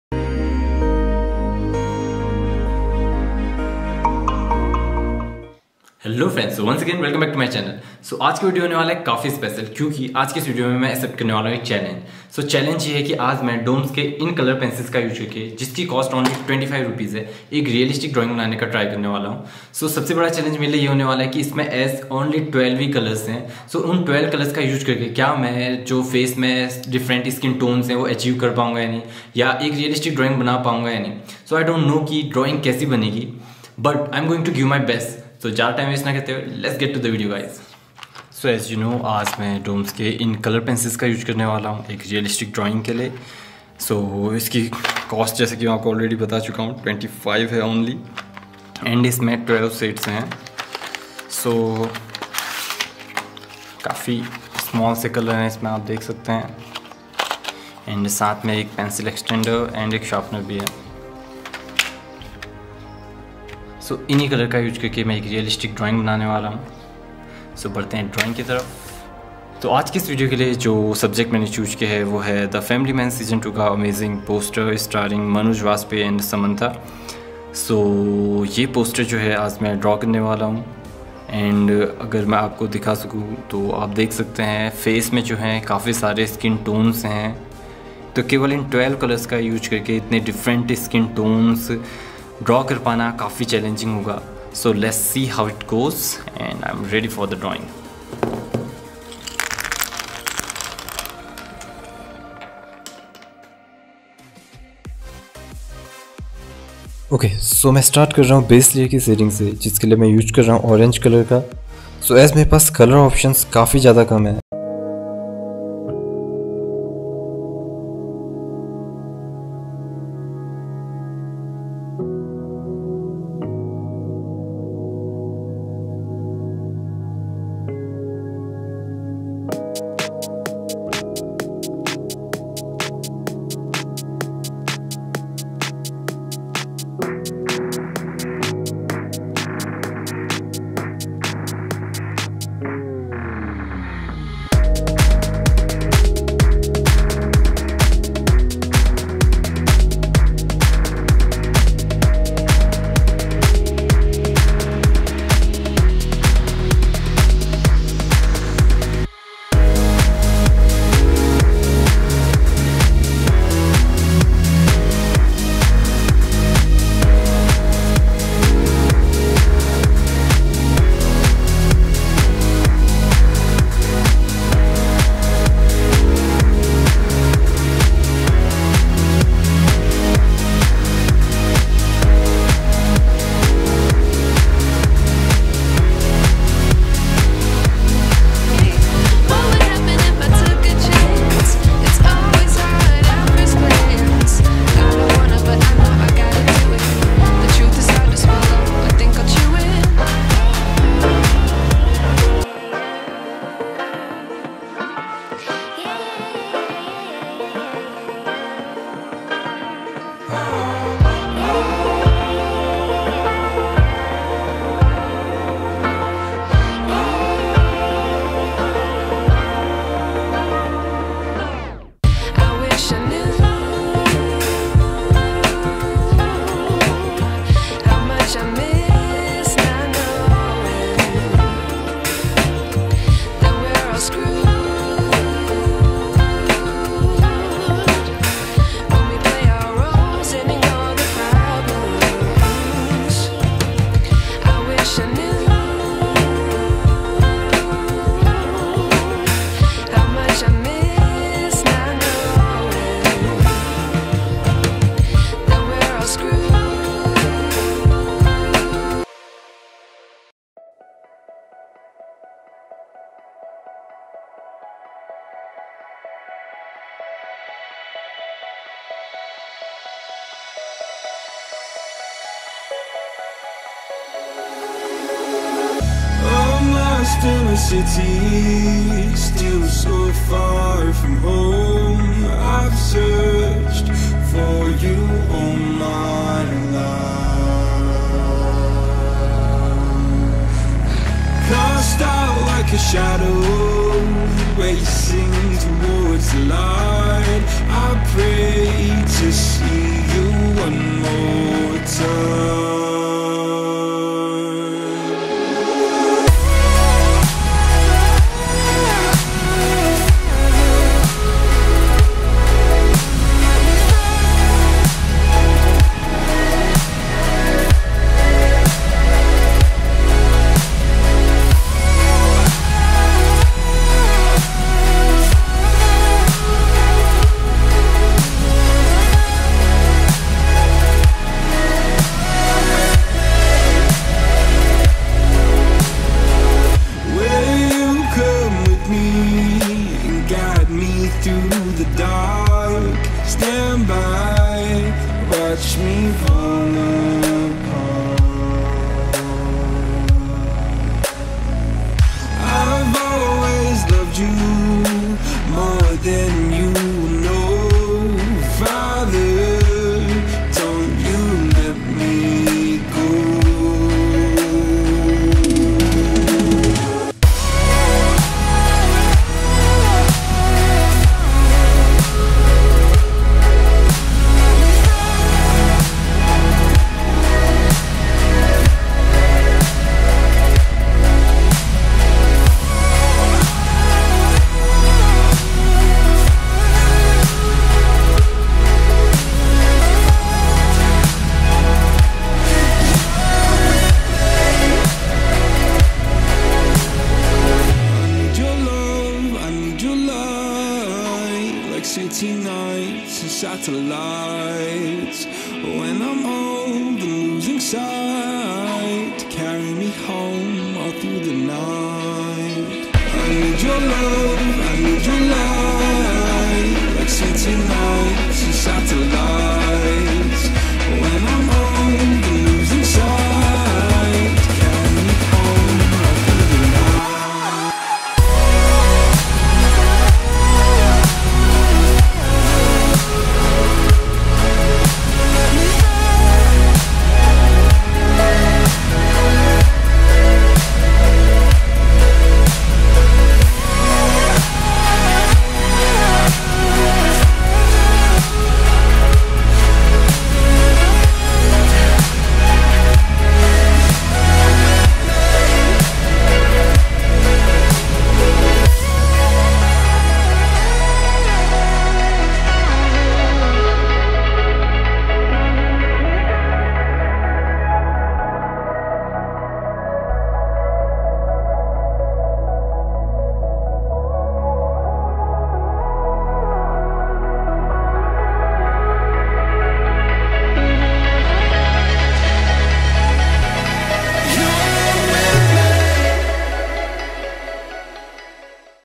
Realistic drawing with 20₹ doms color pencils 😱 // the family man 2 poster drawing
Realistic drawing using doms color pencils costing only 25 ₹. And  these color pencil pack contains only 12 colors, so it will be a challenge to draw all realistic skin tones using these pencil colors.